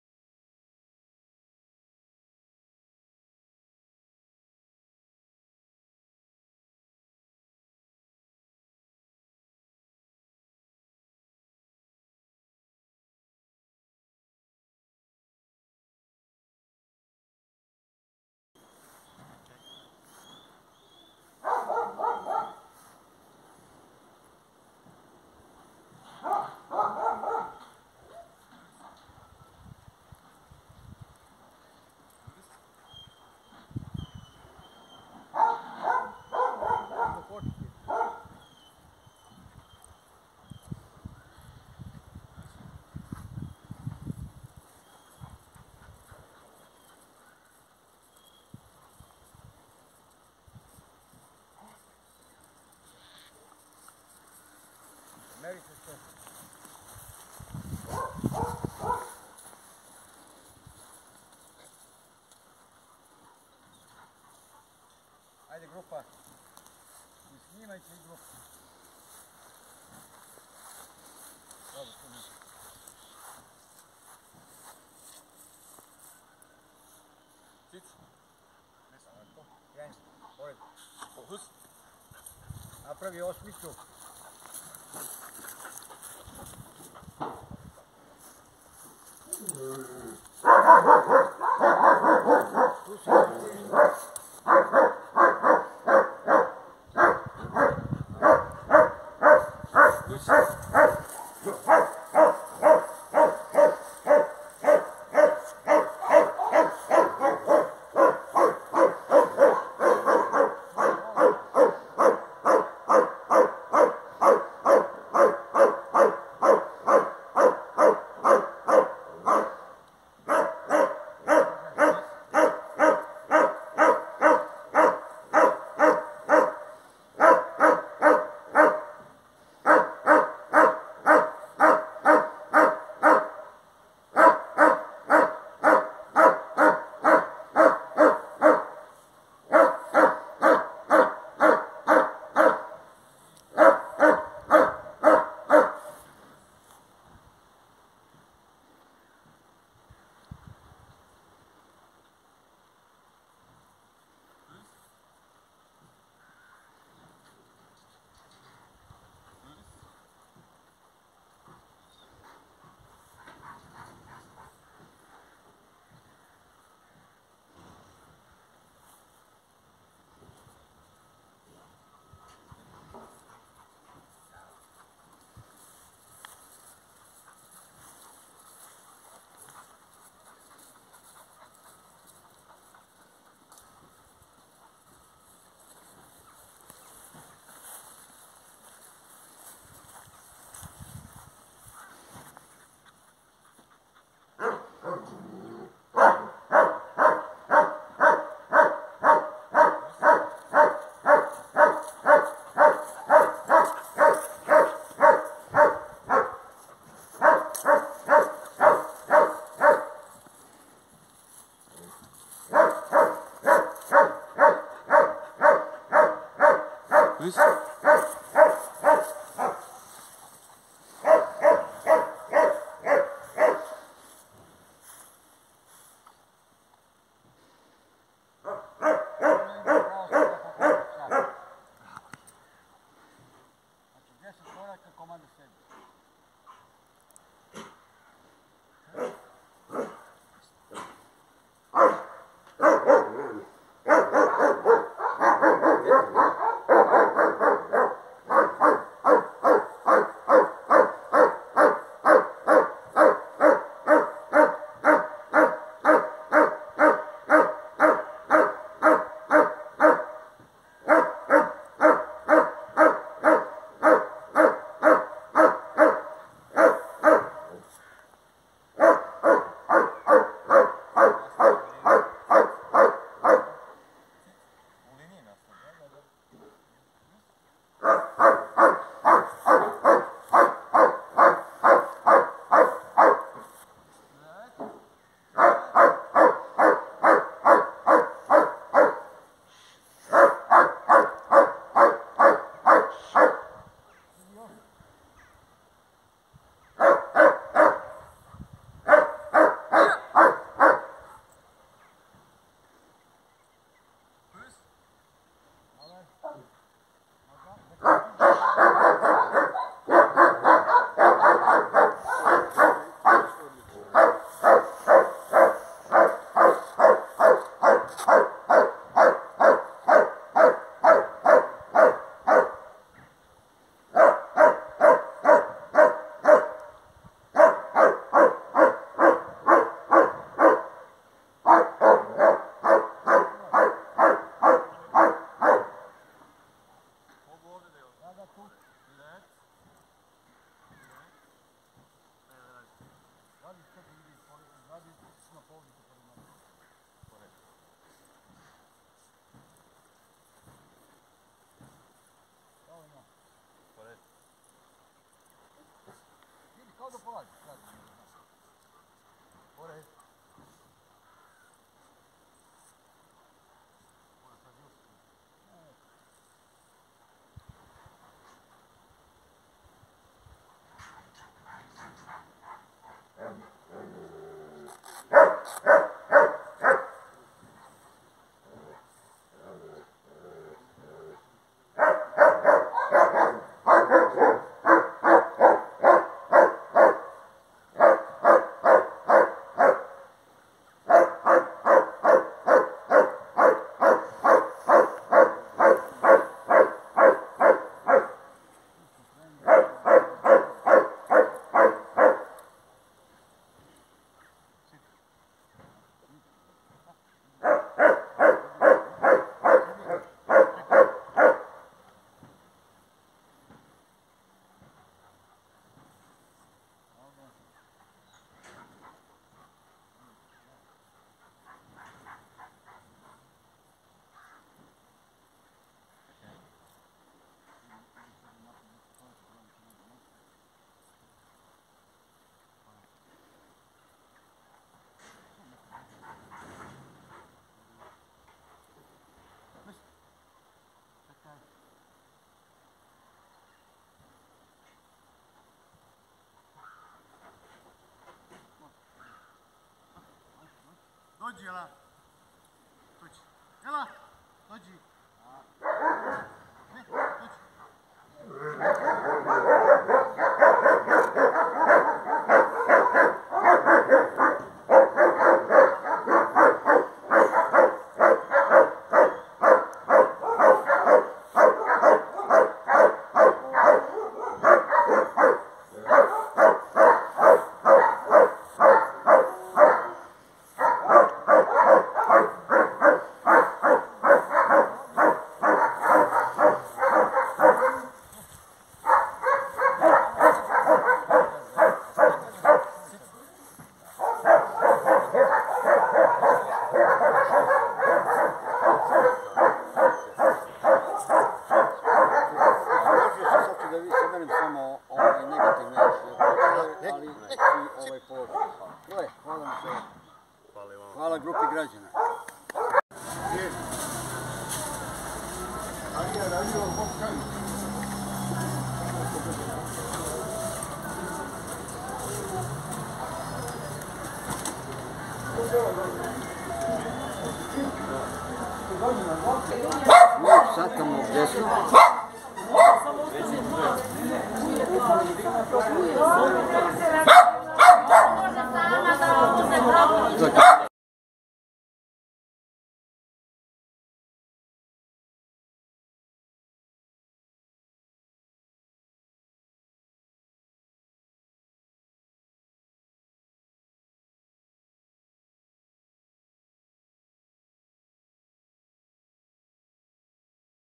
Geli von Galovica BH/VT
Geli von Galovica (Hunter vom Hollermorgen + Elsa von Galovica)
BH/VT @ Kinološko društvo "KG-2012", Kragujevac
sudija: Zoran Miloradović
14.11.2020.